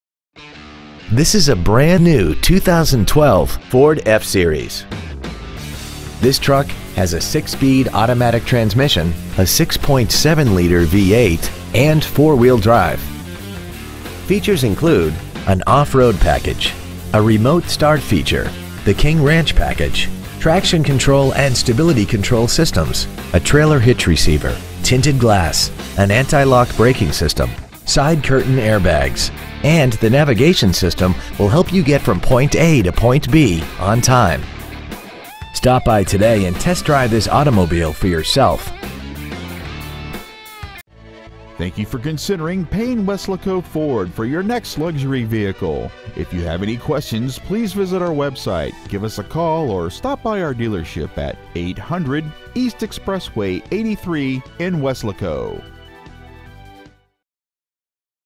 2012 Ford Ford F-Series. Weslaco TX
Year : 2012
 Make : Ford
 Model : Ford F-Series.
 Engine : 6.7L V8 Diesel
 Trans . : Automatic 6-Speed
 Exterior : Autumn Red

 Interior : Adobe w/Unique King Ranch Leather 40/Console/40 Fr
 Stock : EC00426

 800 E. Exp. 83